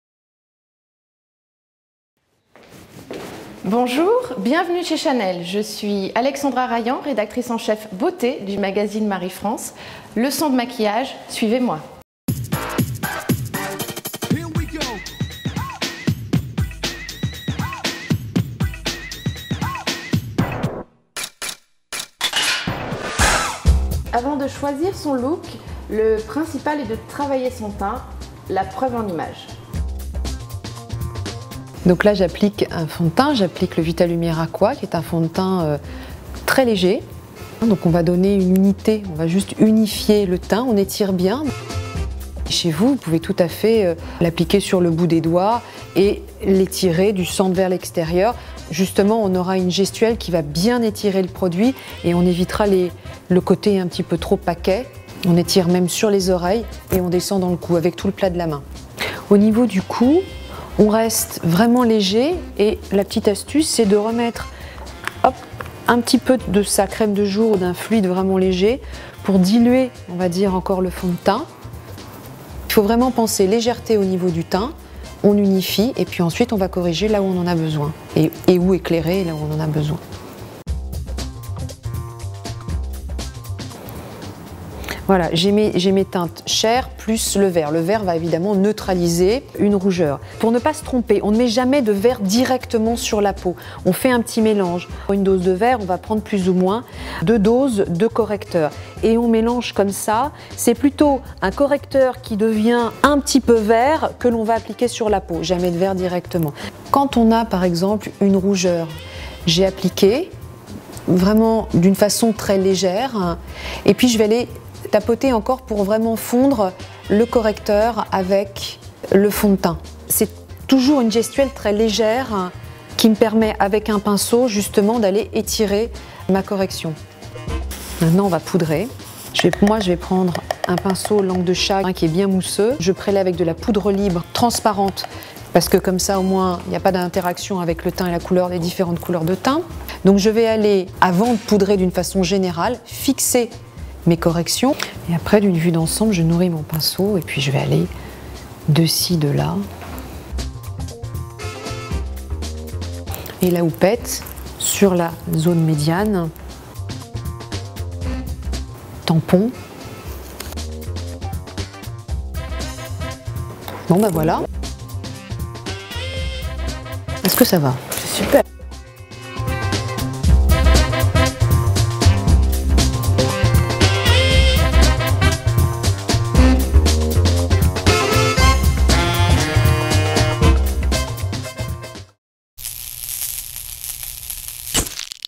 Tuto teint parfait by Chanel
En exclusivité, le tuto pour un teint parfait avec Claire Blavet, make-up artist Chanel.